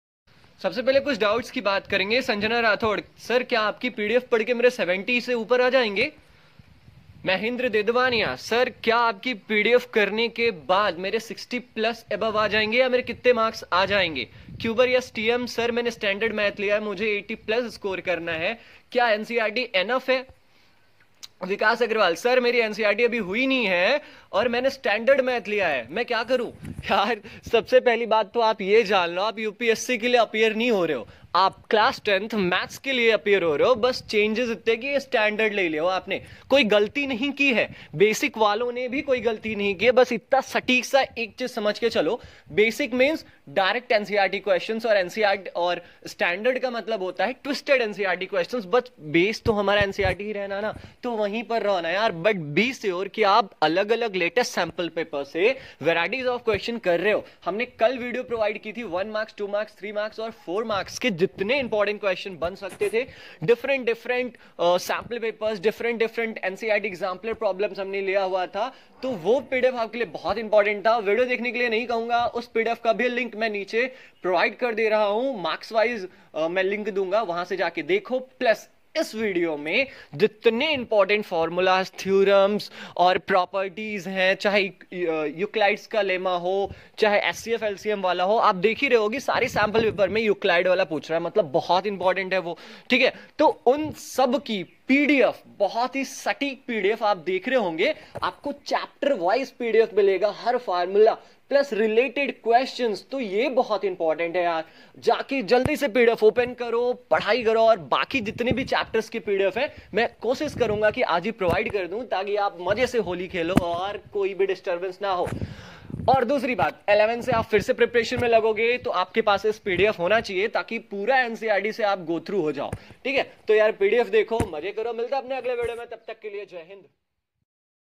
Class 10 Maths (2020) | All important Formulas, Theories, Properties and Important questions | CBSE
Class10Maths(2020) | AllimportantFormulas, Theories, Properties and Important questions | CBSE

This PDF is very important for both STANDARD+BASIC students

All important pdf links :-